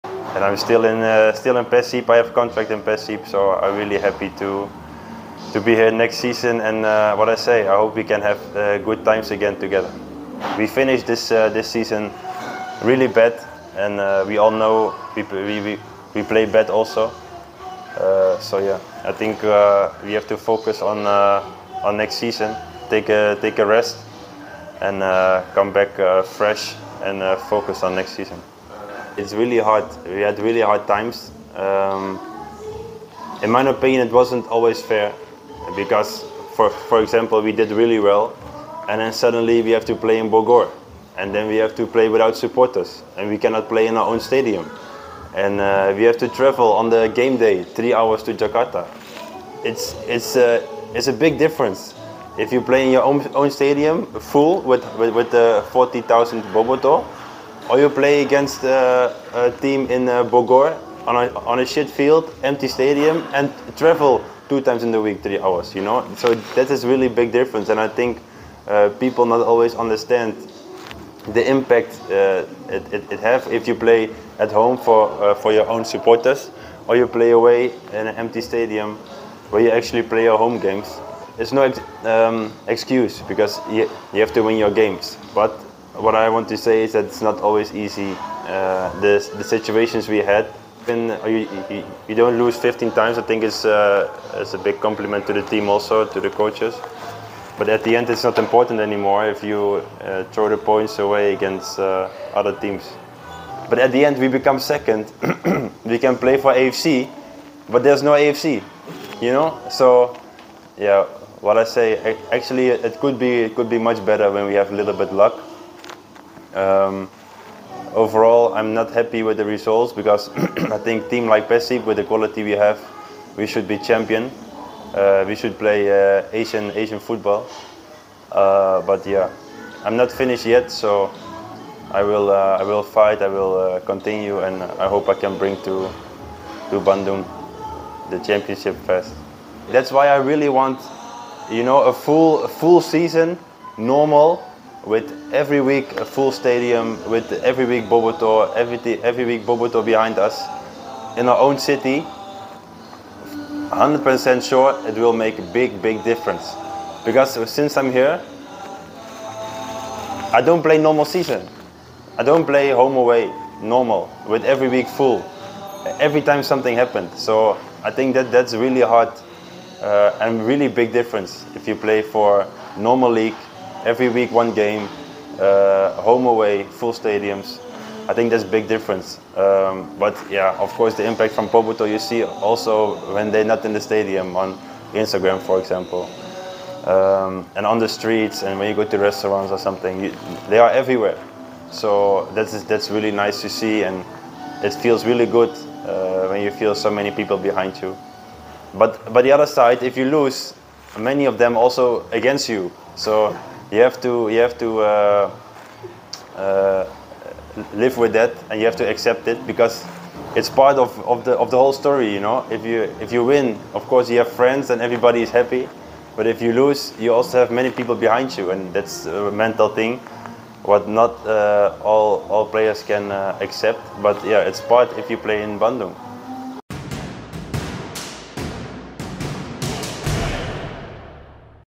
"Bukan Cari-Cari Alasan, Tapi Situasi Kami Tidak Mudah" - Nick Kuipers Evaluasi Liga 1 & Persib
Nick Kuipers mengeluhkan situasi Liga 1 musim lalu yang tidak normal sekaligus mengevaluasi performa Persib Bandung. 🗣️